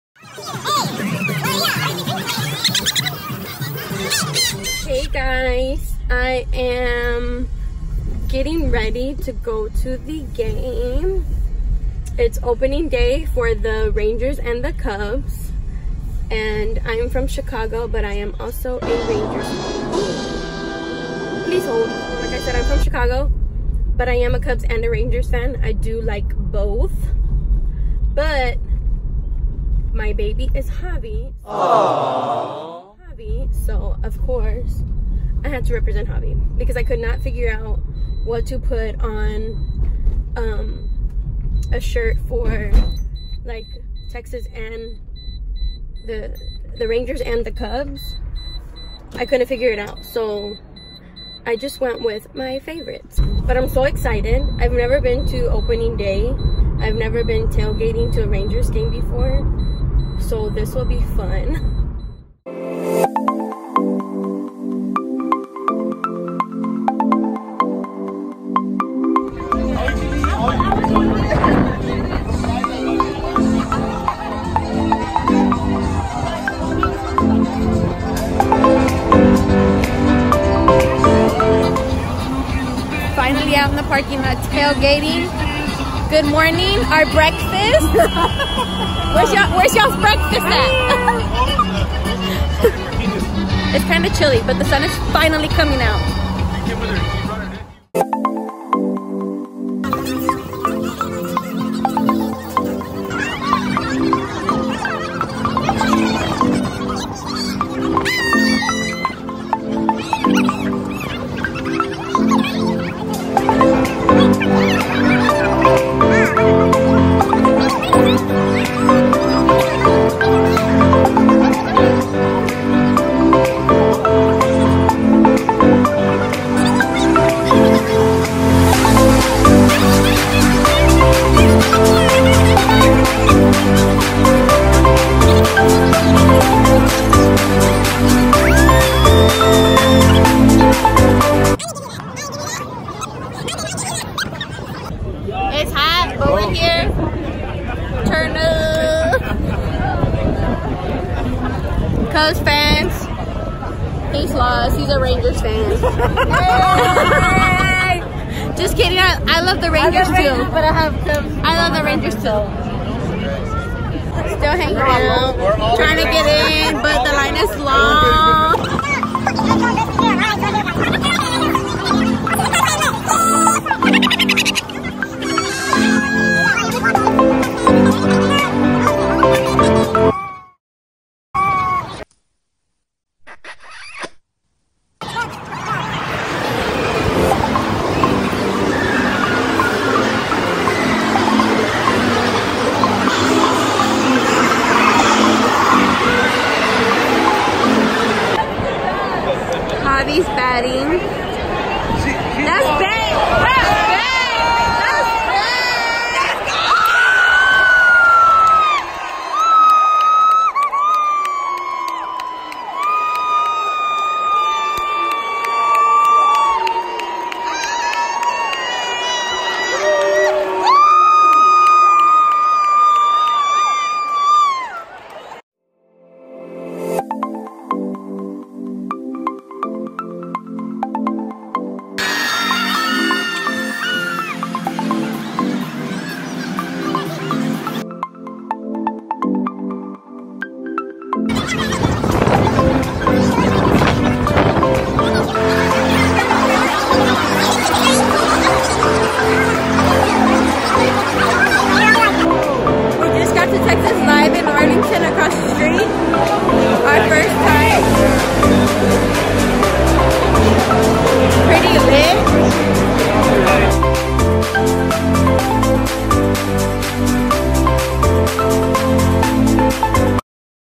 FIRST TIME TAILGATING!!!!
This was my first time tailgating at a game! It was opening day for the rangers vs the cubs! I hope you enjoy!

XxBTStephhxX

Stephh8710